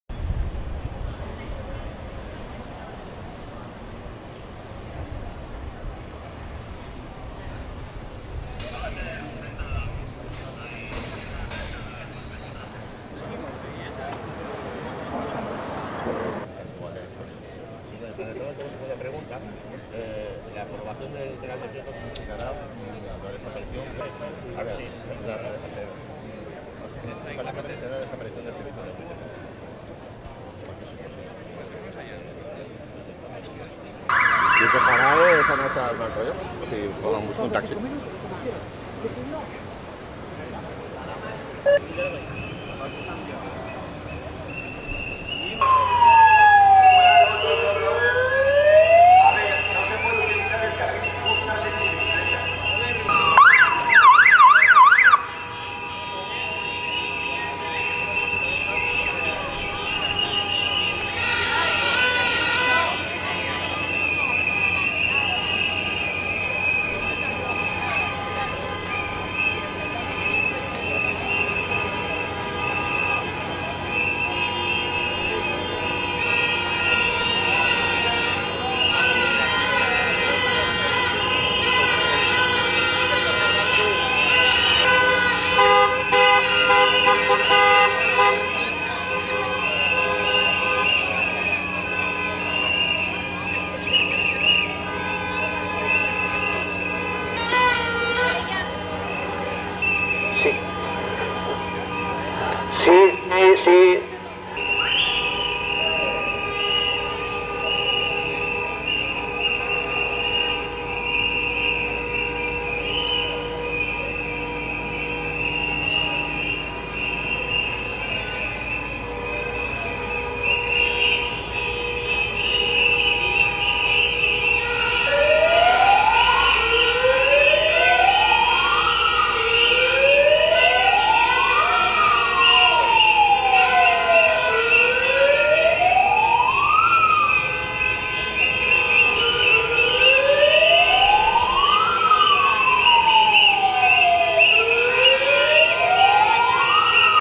VTC marchan en Barcelona contra el decreto ley del Govern
Una cincuentena de vehículos de transporte con conductor (VTC), convocados por el Sindicato Libre de Transporte (SLT), marchan este martes en Barcelona contra el decreto ley de la Generalitat para regular los servicios urbanos a partir del 1 de octubre.